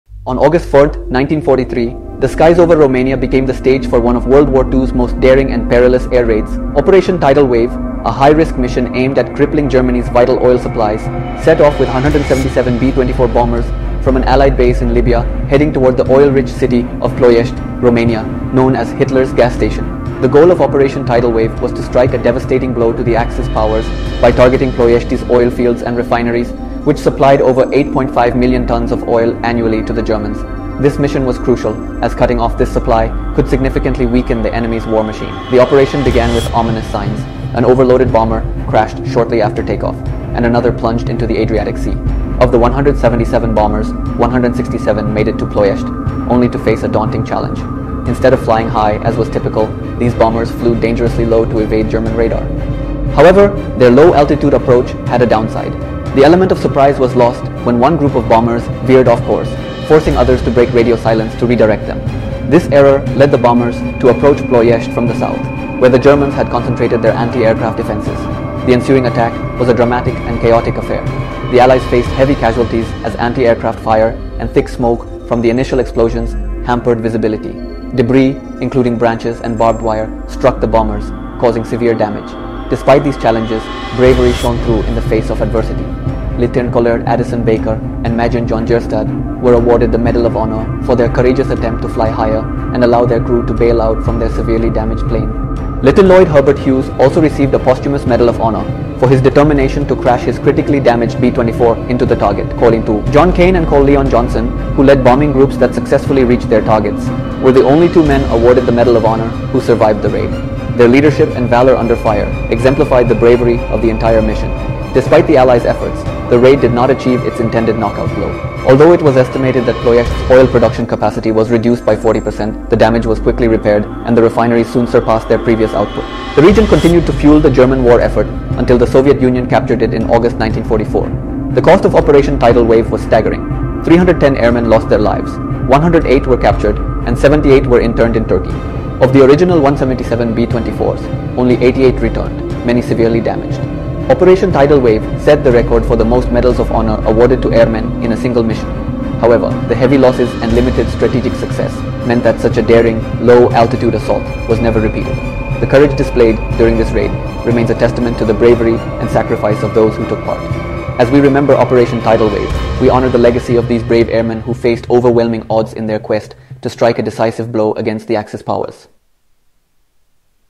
Operation Tidal Wave: The Bold WWII Air Raid That Targeted Hitler’s Fuel Supply
On August 1, 1943, 177 B-24 bombers took to the skies in one of the most audacious missions of World War II: Operation Tidal Wave. Their target? The oil refineries in Ploiești, Romania—Hitler's vital fuel supply. This high-stakes air raid was designed to cripple the German war machine, but it came at a tremendous cost. The mission was marked by immense bravery, devastating losses, and unwavering determination. Despite the overwhelming challenges, the courage and sacrifice of the airmen continue to inspire to this day. Watch the full story of this pivotal operation and honor the heroes who faced impossible odds.